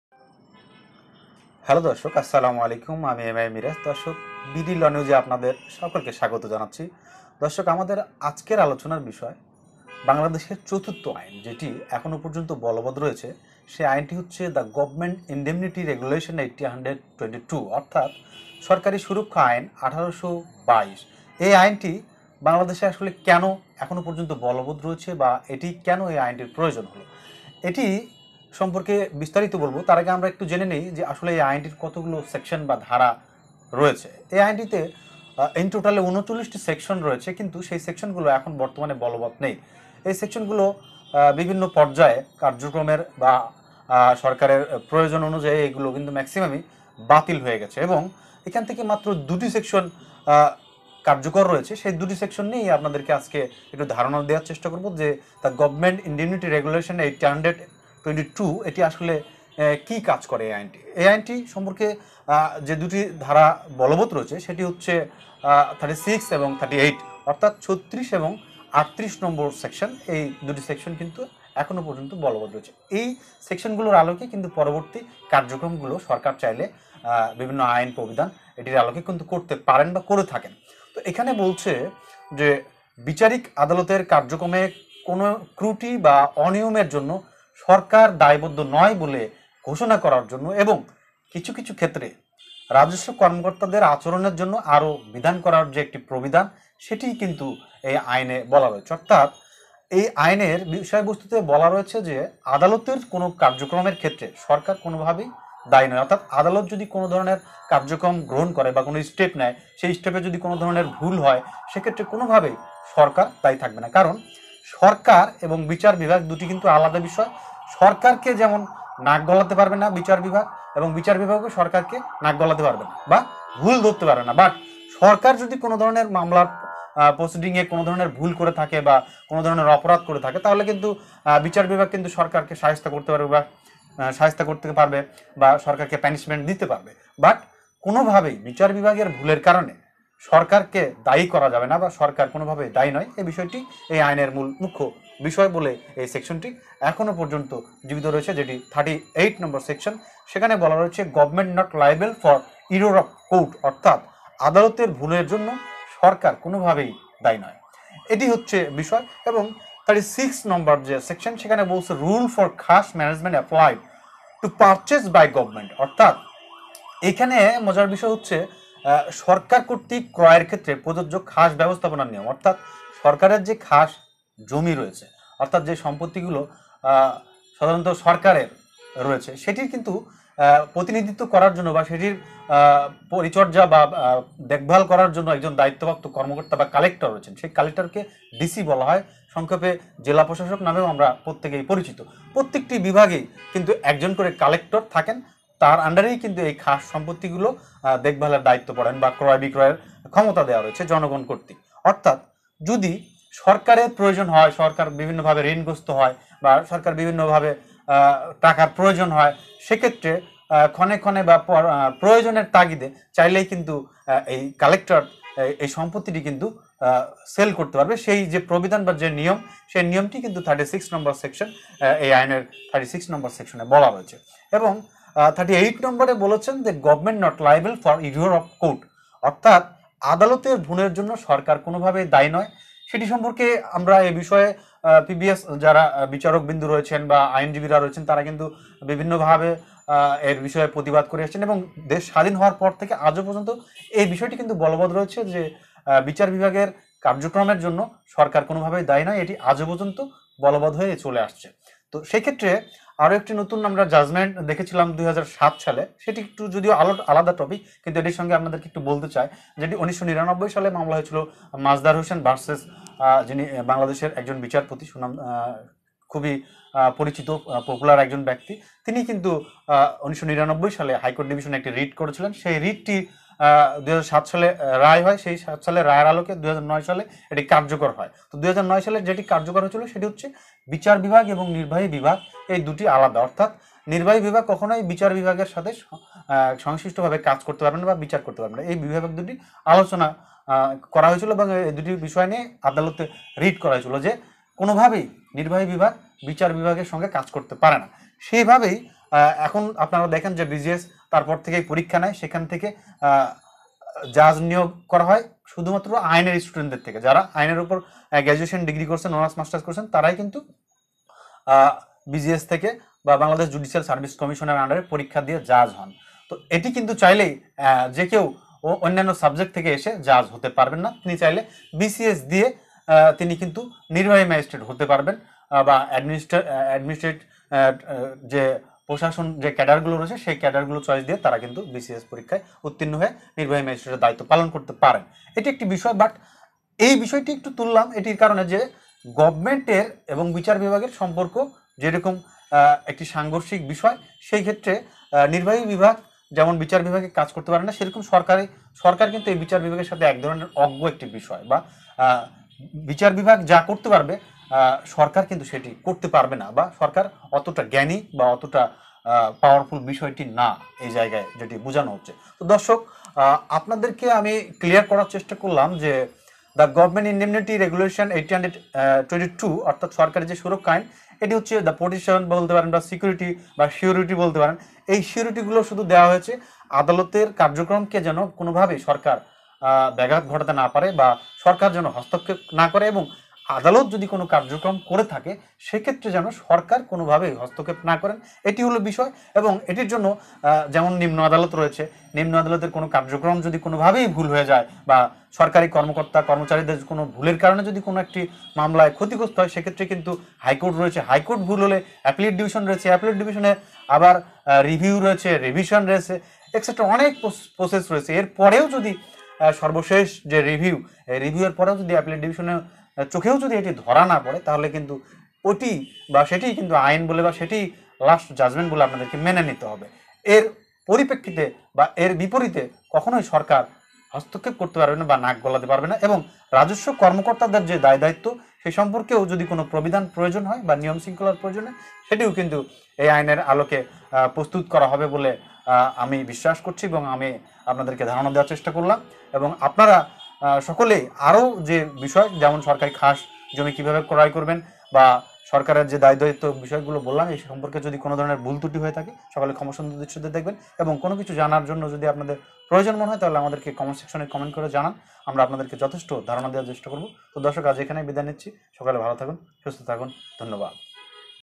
আদালতের ভুলের জন্য সরকার দায়ী নয় ।। Government not liable for errors of Courts
facebook profile of Adv. A.B.M. Shahjahan Akanda Masum

THE GOVERNMENT INDEMNITY REGULATION, 1822

বিচার আদালতের কার্যক্রমে কোনো ত্রুটি বা অনিয়মের জন্য সরকারকে দায়বদ্ধ নয় বলে ঘোষণা করার জন্য এবং কিছু ক্ষেত্রে রাজস্ব-কর্মকর্তাদের আচরণের জন্য আরও বিধান করার জন্য একটি প্রবিধান।

(১) আদালতের ভুলের জন্য সরকার দায়ী নয়
(২) সরকার কর্তৃক ক্রয়ের ক্ষেত্রে প্রযোজ্য খাস ব্যবস্থাপনার নিয়ম